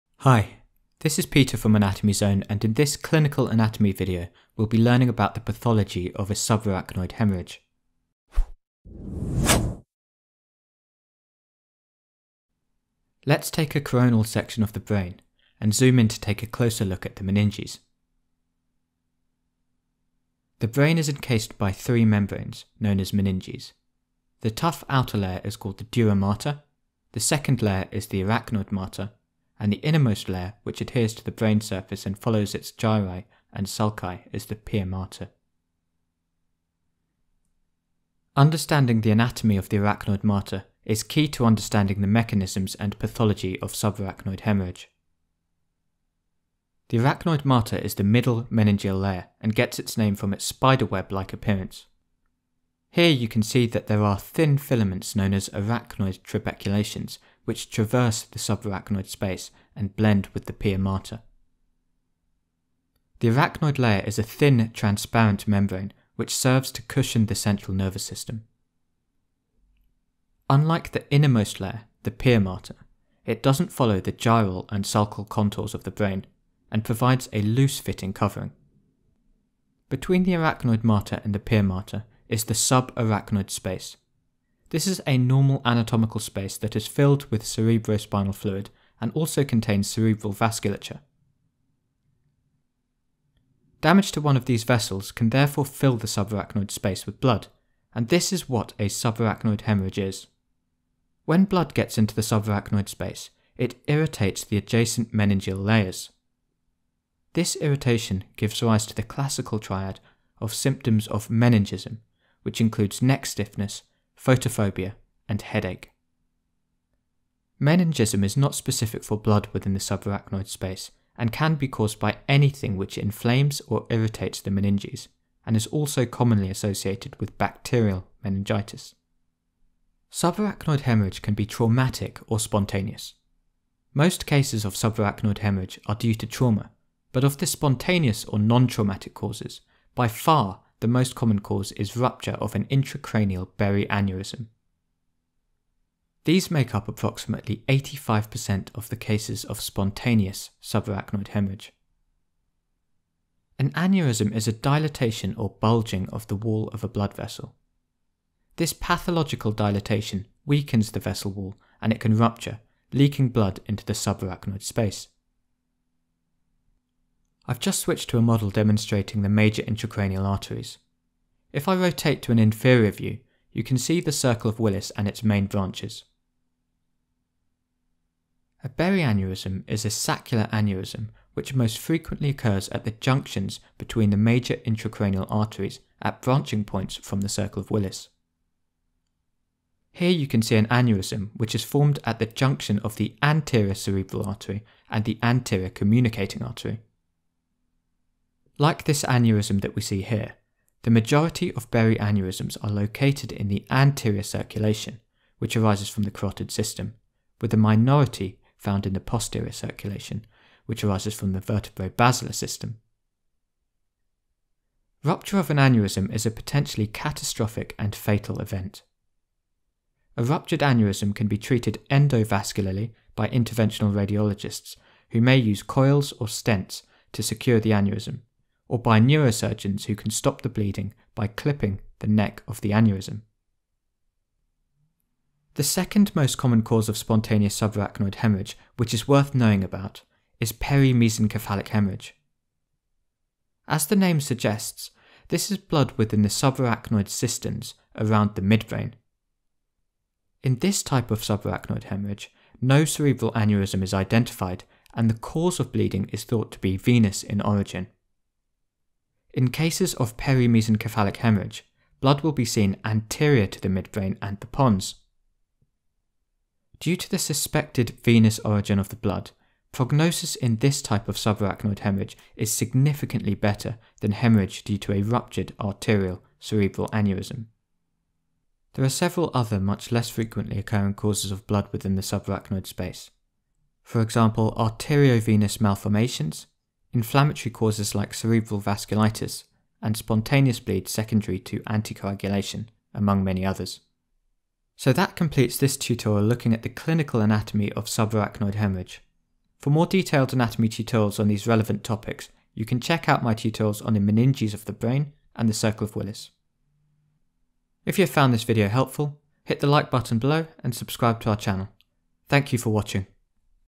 What is Subarachnoid Haemorrhage? | Clinical Anatomy
In this video on subarachnoid haemorrhage we take a look at the relevant anatomy and how this relates to the pathology of subarachnoid haemorrhage. Key concepts, structures and pathologies reviewed:
- Dura mater, arachnoid mater, pia mater
- Subarachnoid space, arachnoid trabeculations
- Berry / saccular aneurysm
- Perimesencephalic haemorrhage

This video tutorial was created using images from our partners at OpenStax College, these images can be downloaded free